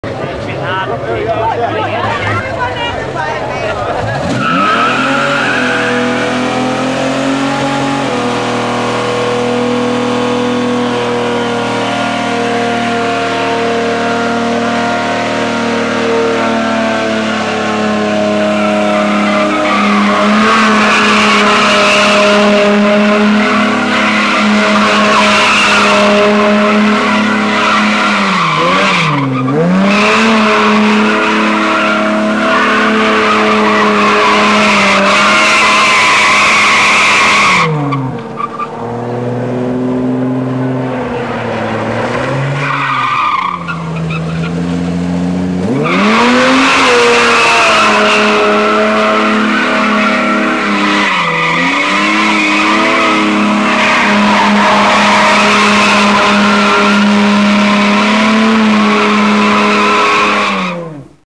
Ford XE 6cyl Falcon Burnout at an Aussie Bush Party
The 6 carburetors on this Ford provide more torque than power, and put on a fine show on the concrete pad.